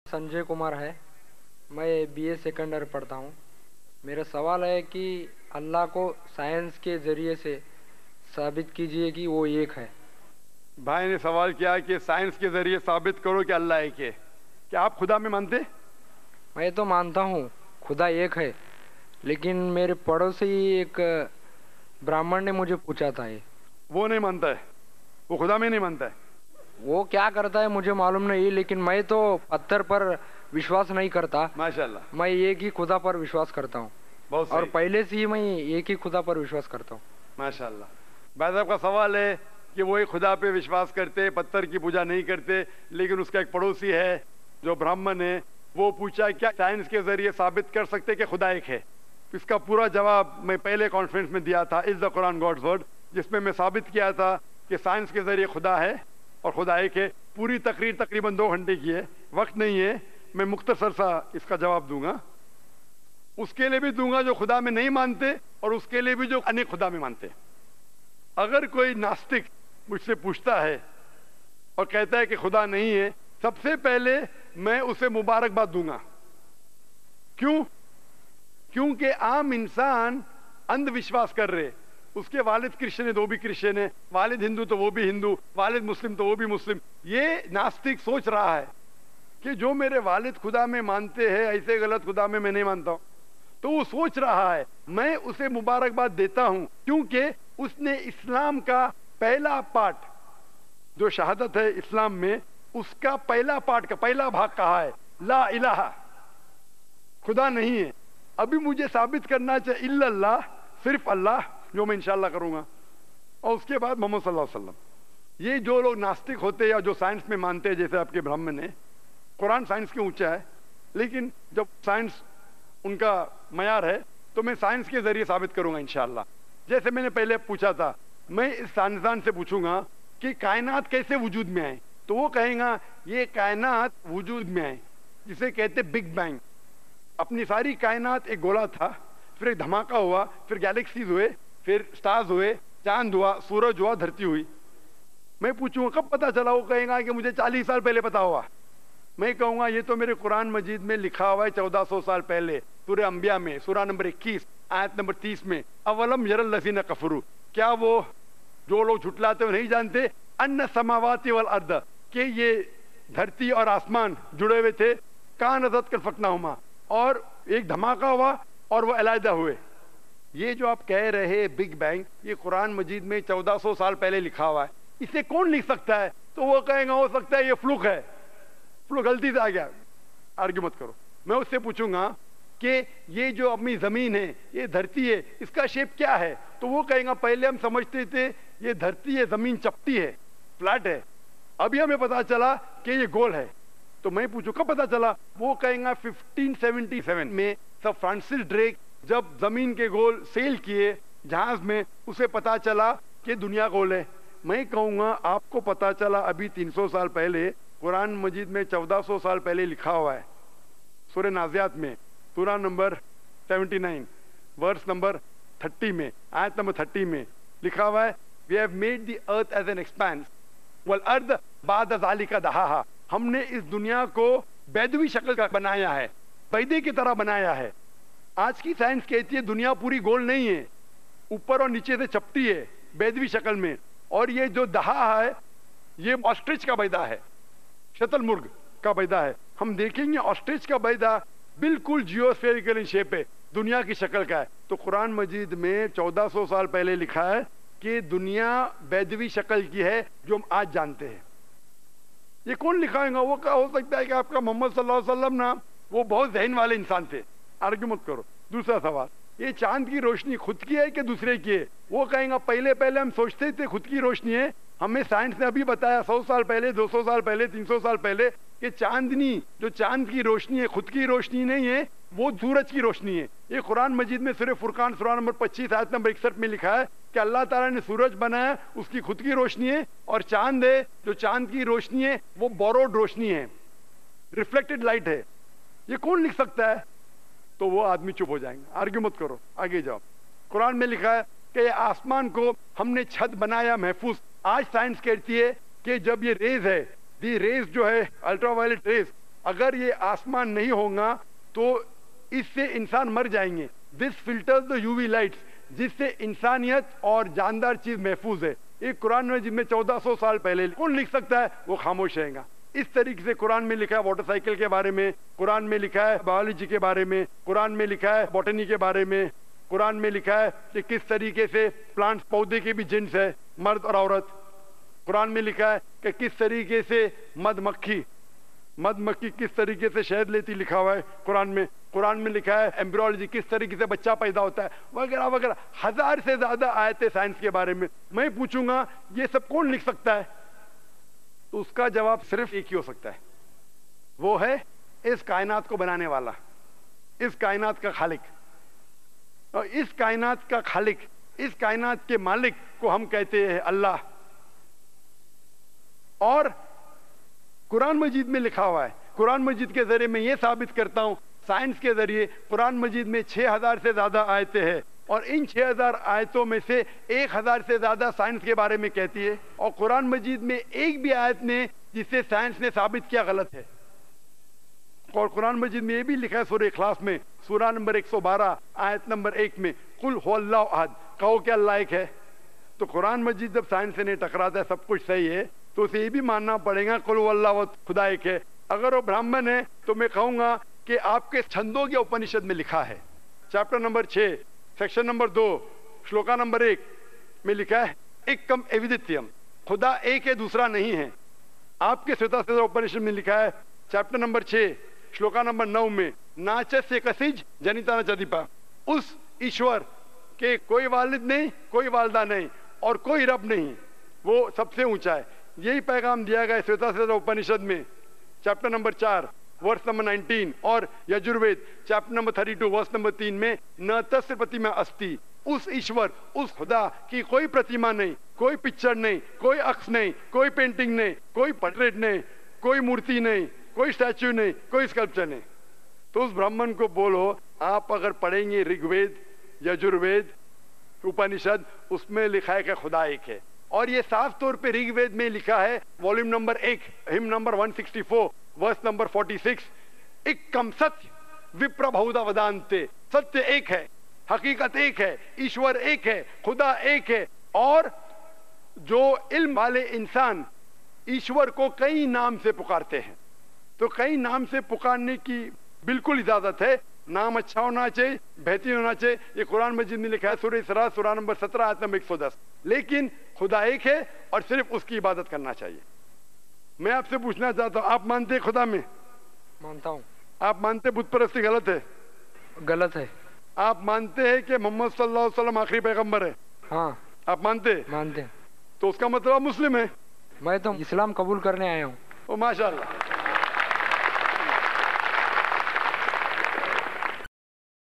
Allah Ko Sceince Ke Zareye Sabith Kijeye? اللہ کو سائنس کے زرے سبیت کیجیے? - Dr. Zakir Naik Jawab
0:00 Intro

Mein Man'tha hu khuda ek hai. Lekein mera Padosi nahi man'tha.
Muje Ek bhraman ne poocha tha.
Woh nahi man'tha?
Woh khya kar'tha hai muje maa'lum nahi! lekein mein tho pattar par vishwaas nahi kar'tha.
Mein tho ek hi khuda par vishwaas kar'tha hu. Aur Peh'le se ek hi khuda par vishwaas kar'tha hu.

1:09 *nastik (ملحد) ko Kaise Khuda mein Vishwaas dila sak'the hai?*
sabse pehle use mein Mubarak pesh karu ga.
Kyu ke Aam insaan Andh vishwaas kar rah'he hai.
Uska Walid christian hai tho woh bhi Christian hai.
Uska Walid hindu hai tho woh bhi Hindu hai.
Uska walid muslim hai tho woh bhi Muslim hai.
Yeh, nastik soch'rha hai!

Jo mera walid man'the hai, wai'se ghalat khuda mine mein nahi man'tha hu.
Mein use Mubarak deta hu kyu ke, usne Islam ka pehla part yani kalimaa "La illaha" kaha.
khuda nahi hai, ab muje saabith kar'na hai illah Allah (sirf Allah) woh mein Insha Allah kar'nga aur uske ba'ad Muhammad S.A.W ke bare mein.
Ye Nastik zyada log science mein man'the hai. Lekein Quran science se u'oncha hai.

2:08 *Kainaat kaise Ujood mein Aya?*
Yeh kainaat ek ghola tha. Ek Dhamaka huwa. Us'se galaxy huwe, suraj huwa, chand huwa, dharti huwi.
Mein poochun'ga kab patha chala?
40 saal peh'le patha chala.
mein kahun'ga yeh mere Quran majeed mein 1,400 saal pehle likha huwa hai
Surah_Al_Ambiya, Chapter # 21, verse # 30
Kya woh jhot'late hai woh nahi jaan'the, ke ye aasmaan aur zameen jhoode huwe the aur ek dhamaka huwa aur aalida huwe.
woh keh sak'tha hai ye Flock hai ya ghalthi se aagaya.

3:01 *dharti ka shape kya hai?*
pehle hum samaj the te. Yeh dharthi chapti hai (flat hai). Abhi hame patha chala ke ye ghol hai.
1577 jab Francis_Drake zameen ke ghol kiya. ja'nha se use patha chala ki duniya ghol hai.
surah_Al_Naziyat, surah 79, verse # 30
Aap deke ghe duniya شترمرغ (ostrich) ki Ande tarah upar and niche se chapti hai.
Quran majeed mein 1,400 saal pehle likha huwa hai.
kuch log kahn'ge muhammad S.A.W thode intelligent insaan the.

4:19 *Chand ki roshni kud ki hai ya dus're ki hai?*
pehle hum soch'the the chand ki roshni kud ki hai.
Hame 100 saal patha chala, chand ki roshni kud ki nahi borrowed kiya huwa hai.
Chand ki roshni sooraj se liya huwa roshni hai (borrowed light).
surah_Al_Furqaan, Chapter 25, Verse # 61
Allah tah'la ne suraj bana ya aur chand ki roshni borrowed roshni hai (reflected light).